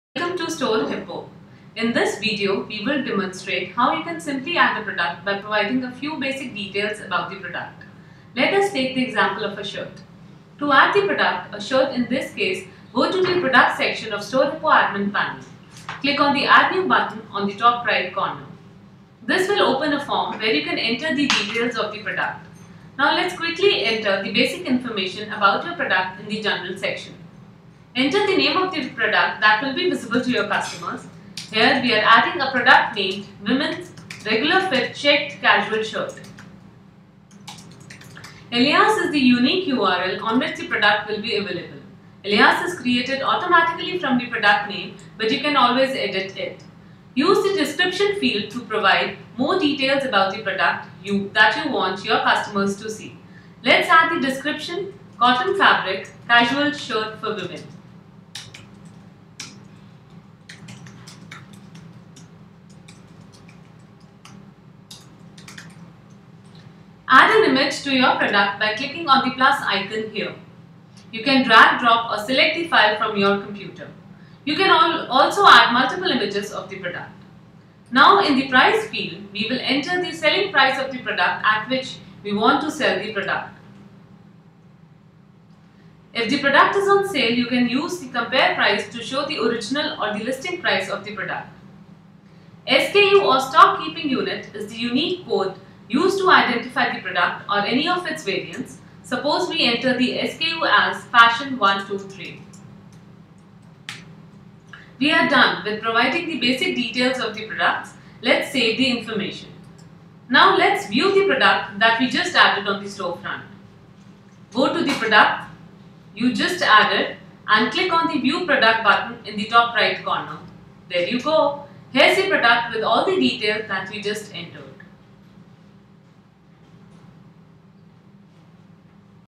StoreHippo: Adding a simple product
Learn how to add products with basic details on your e-commerce store powered by StoreHippo. Refer the link to free tutorials on Adding Products to stores